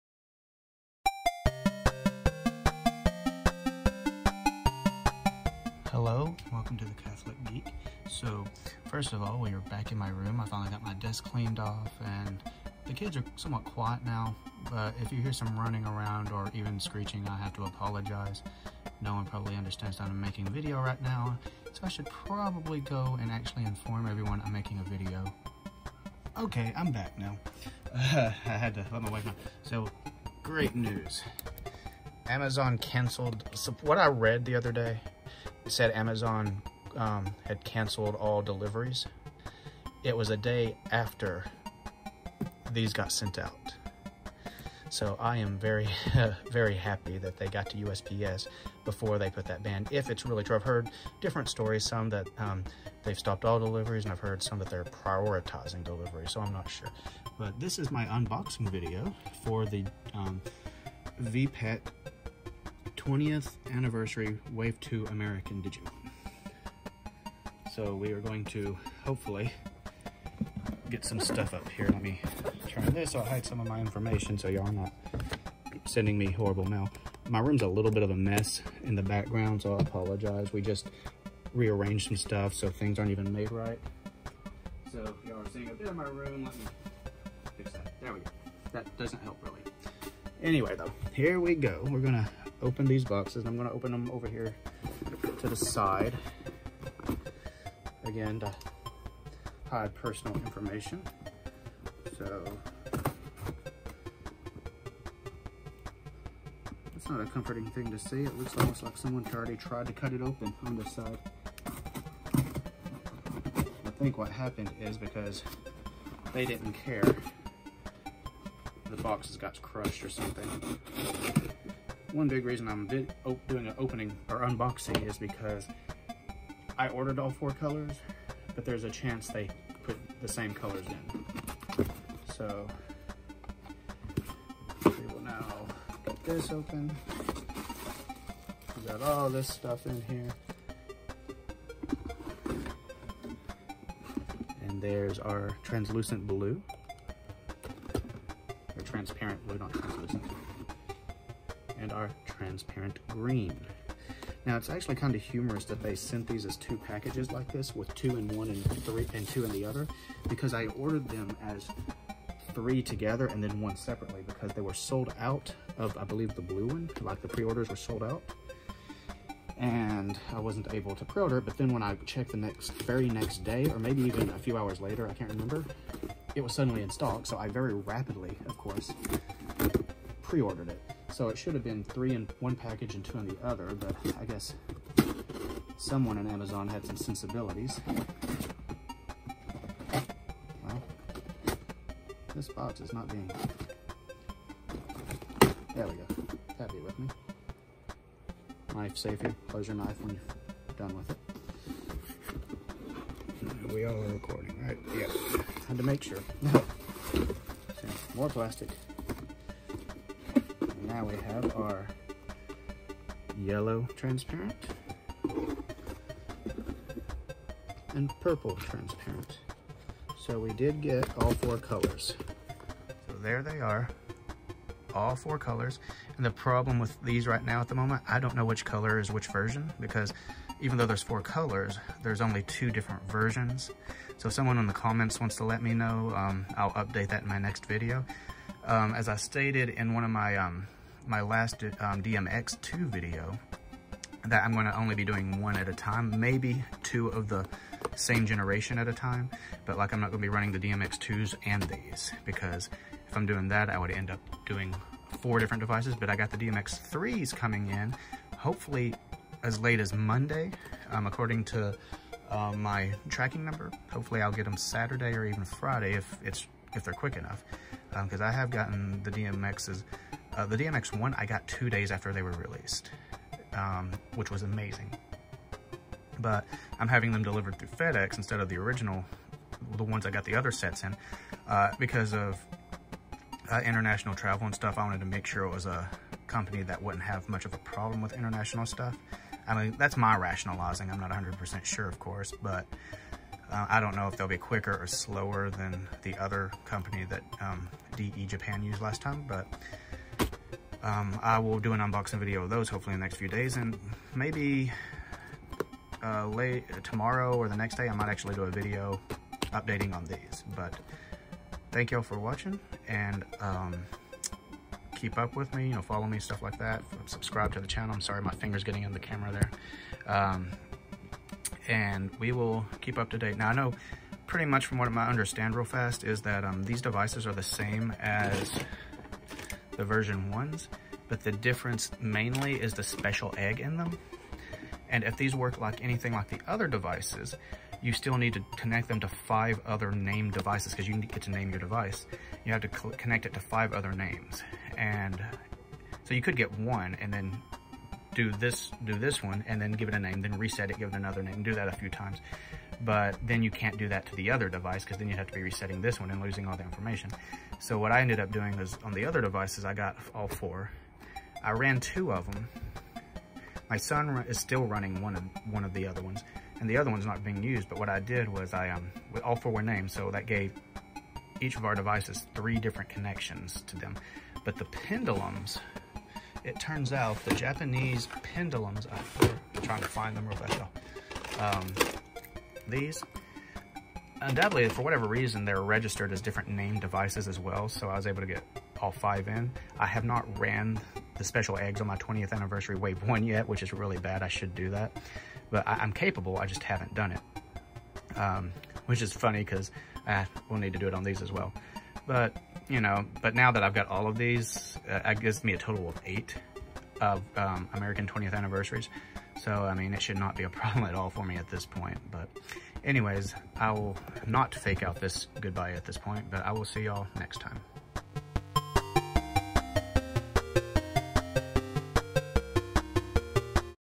Digimon 20th Anniversary American V-Pet Wave 2: Unboxing
I finally got all of Wave 2. Watch as I inbox them!!!
From now on, I will refer to them as AV20-2s.